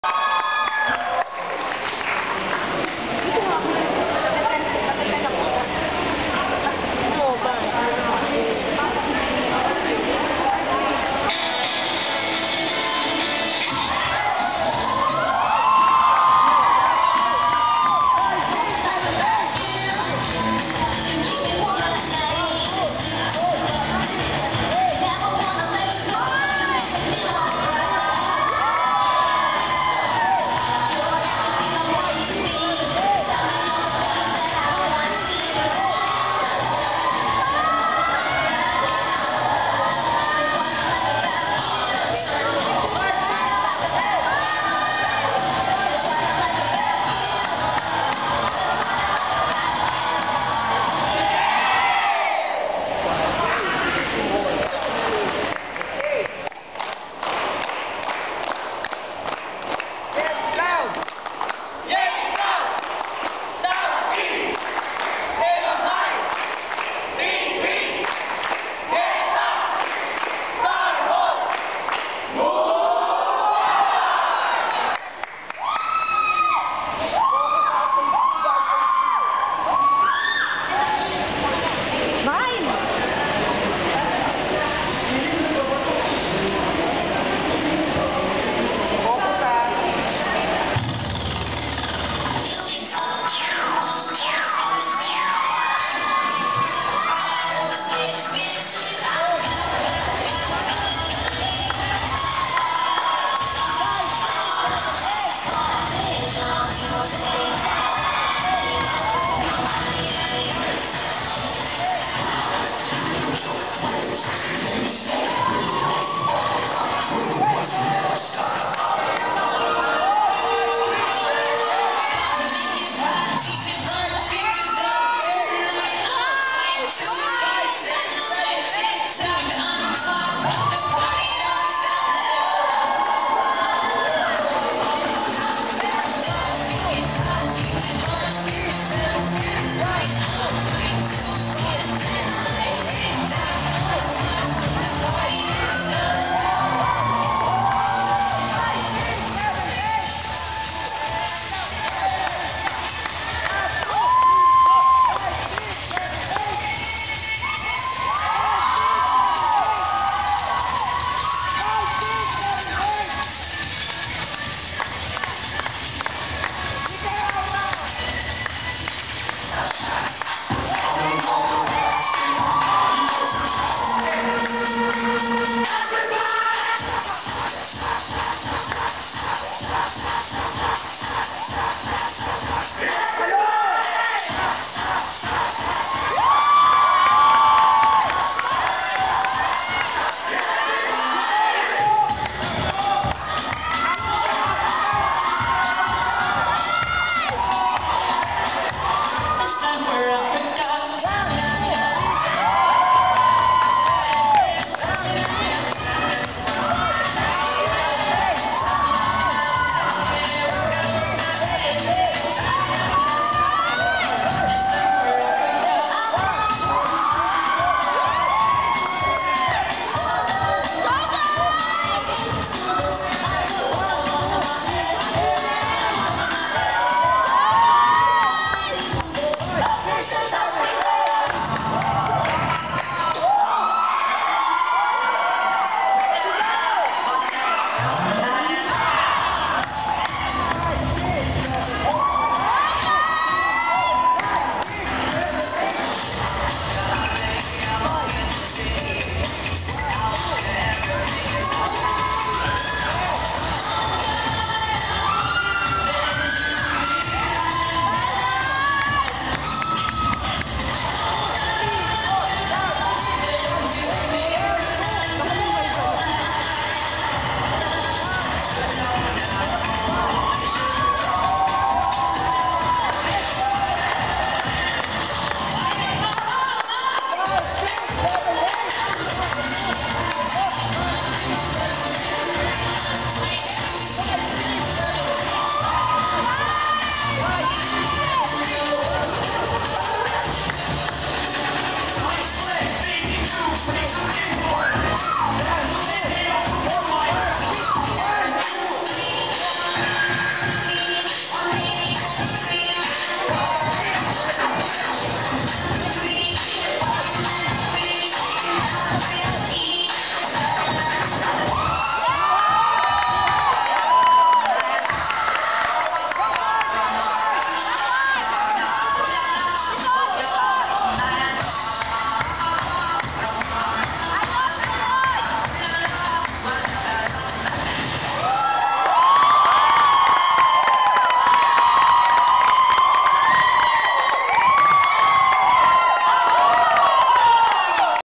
Mall South Cheerdance Presentation
Starmall Sportsfest and Outing 2010

Island Cove Kawit, Cavite